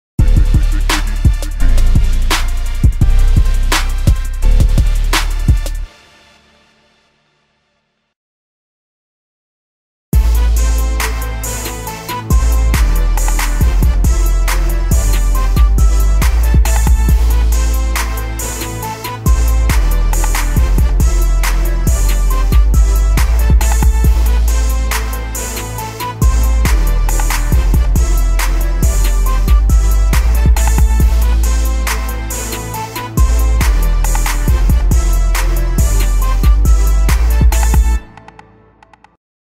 Figgy Beats - Trailer - May 13th - FL Studio 10
I'm going hardcore on this beat, putting A LOT of time into it.